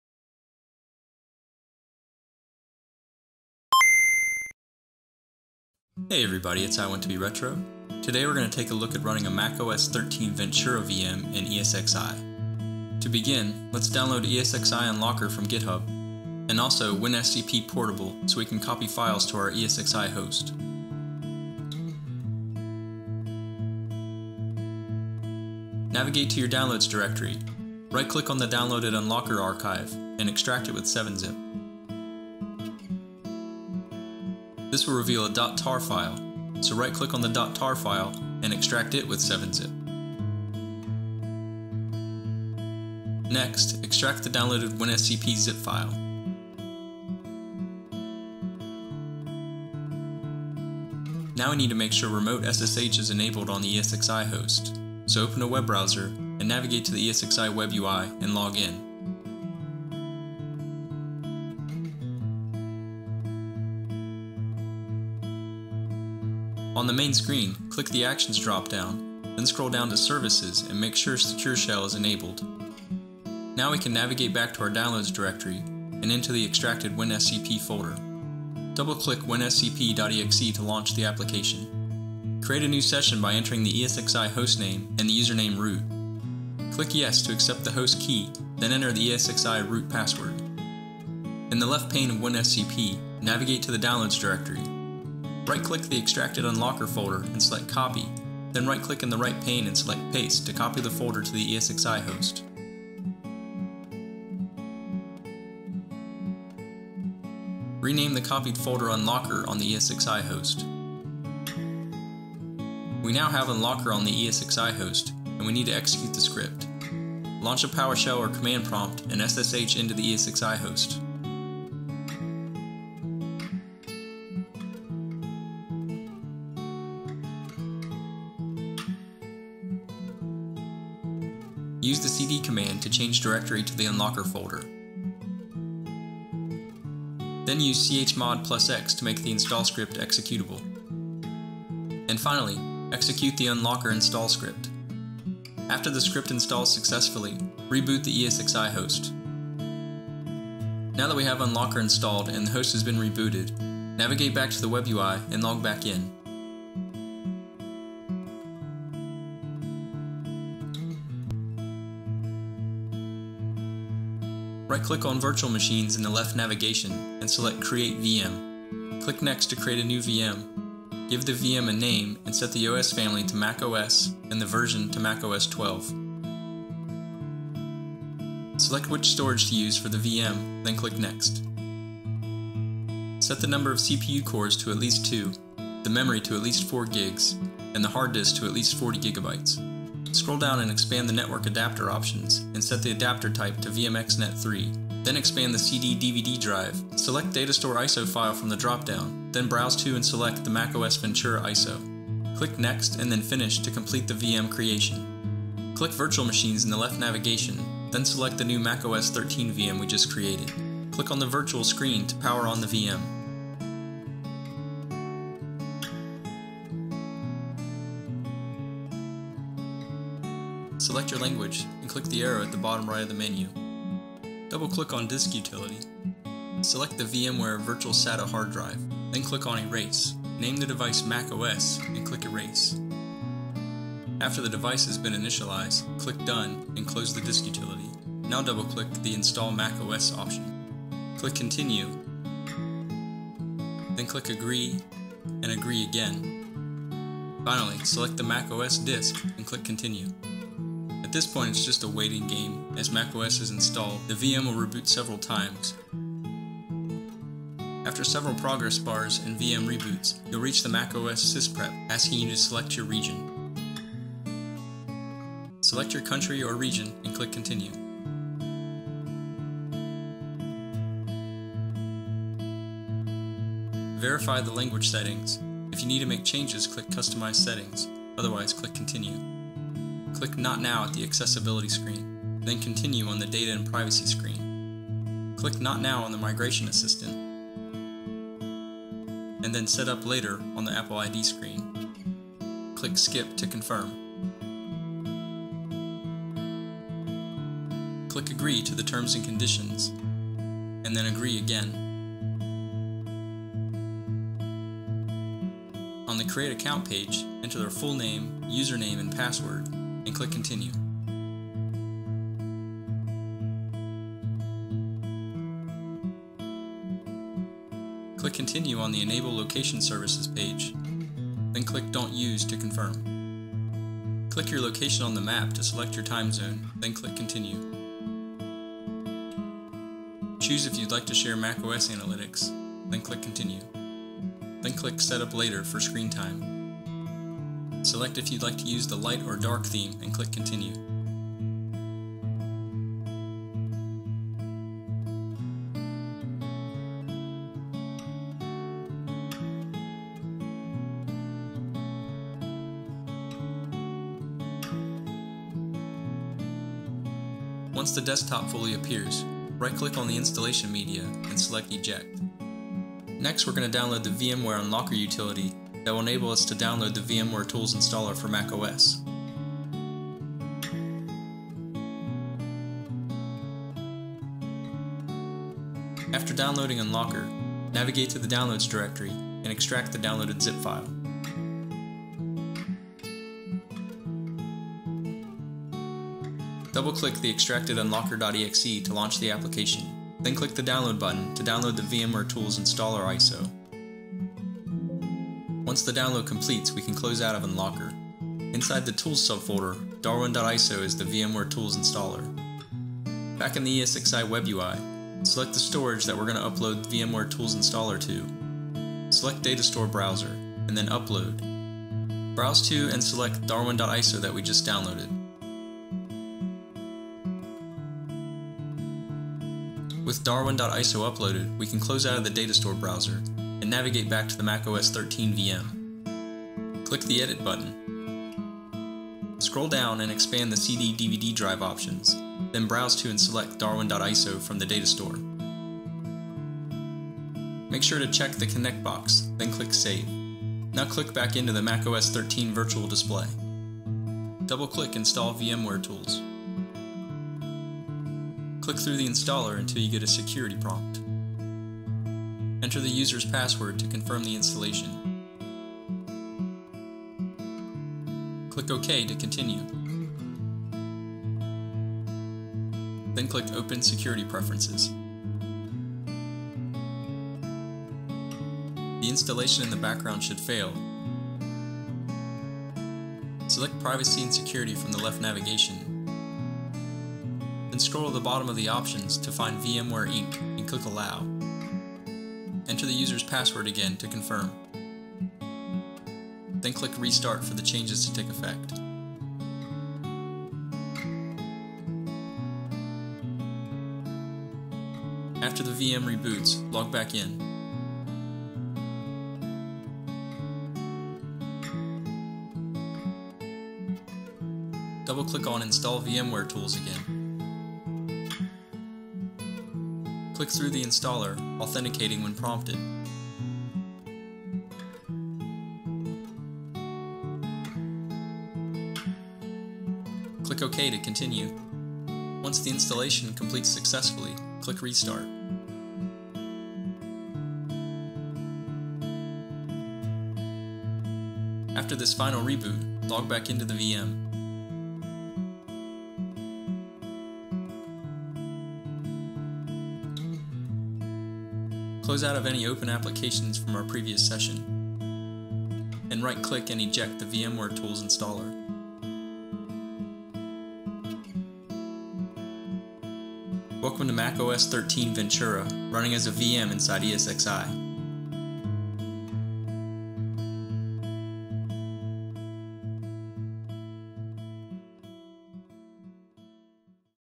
Running a MacOS 13 Ventura VM in VMware ESXi
Downloads and Configuring ESXi
01.
02.
   04. Extract the downloaded Unlocker for VMware ESXi
   05. Rename the extracted folder unlocker
   06. Log into the ESXi web UI
   07. Enable the SSH service by selecting Actions ≫ Services ≫ Enable Secure Shell (SSH)
   08. Extract WinSCP and run the executable
   09. Connect to the ESXi host IP address via WinSCP
   10. Copy the extracted Unlocker folder to the ESXi host /root directory
   11. Connect to the ESXi host via SSH
   12. Run the following commands to install ESXi Unlocker
         # change directory to unlocker
         cd /unlocker
         # make the install script executable
         chmod +x ./esxi-install.sh
         # run the install script
         ./esxi-install.sh
         # reboot esxi host
         reboot

Creating the Ventura VM
   01. Log into the ESXi web UI
   02. In the left navigation pane, right click on Virtual Machines ≫ Create/Register VM
   03. Select Create a new virtual machine ≫ Click Next
   04. Name the VM MacOS 13 ≫ Set the Guest OS family to Mac OS and Guest OS Version to Apple MacOS 12 (64-bit) ≫ Click Next
   05. Select the storage datastore to use ≫ Click Next
   06. Set the Memory to at least 4096, Hard disk 1 to 50 GB or more
   07. Expand Network ≫ Set the Adapter Type to VMXNET 3
   08. Expand CD/DVD Drive 1 ≫ Make sure Datastore ISO file is selected ≫ Click Browse next to the CD/DVD Media field
   09. Click Upload ≫ Browse to and select the Mac OS installation ISO
   10. Once the upload completes, click the Mac OS ISO to highlight it ≫ Click Select
   11. Back on the Hardware customization screen click Next
   12. Review the summary provided and click Finish Click the new MacOS 13 VM Click Power on at the top of the page

Installing MacOS Ventura
   01. After an initialization sequence the MAC OS Setup should start
   02. Select a Language ≫ Click the next arrow
   03. Select Disk Utility
   04. Select the VMware Virtual SATA Hard Drive Media ≫ Click Erase
   05. Name the drive MacOS ≫ Set the Format to APFS ≫ Click Erase
   06. Click Done ≫ Close Disk Utility
   07. Click Install macOS Ventura
   08. Click Continue ≫ Click Agree ≫ Click Agree again
   09. Select the MacOS disk ≫ Click Install
   10. Wait while Mac OS installs files, the VM will reboot several times
   11. Select your Country ≫ Click Continue
   12. Confirm your languages and keyboard layout ≫ Click Continue
   13. Click Not Now on the Accessibility screen
   14. Click Continue on the Data & Privacy screen
   15. Select Not Now on the Migration Assistant screen
   16. Select Set Up Later and then Skip on the Apple ID screen
   17. Click Agree ≫ Agree again
   18. Enter a name, user name, password ≫ Click Continue
   19. Click Customize Settings
   20. Click Continue ≫ Select Use or Don't Use for Location Services
   21. Pick a time zone ≫ Click Continue
   22. Choose whether to share analytics with Apple ≫ Click Continue
   23. Click Set Up Later on the Screen Time screen
   24. Pick a theme ≫ Click Continue
   25. Welcome to MacOS 13 Ventura

Install VMware Tools (optional, but recommended)
01.
   02. Extract the downloaded .zip file
   03. Run Unlocker.exe
   04. Click the Download button under the Tools section
   05. Back in the ESXi web UI select Storage from the left navigation menu
   06. Click the Datastore browser icon at the top of the main content pane
   07. Click Upload ≫ Browse to and select the darwin.iso downloaded by MacOS Unlocker
   08. Once the upload completes click Close to close out of the Datastore browser
   09. Select the MacOS 13 VM ≫ Actions ≫ Edit settings
   10. Attach darwin.iso to CD/DVD drive 1
   11. Inside the VM, double click the mounted VMware tools and run the installer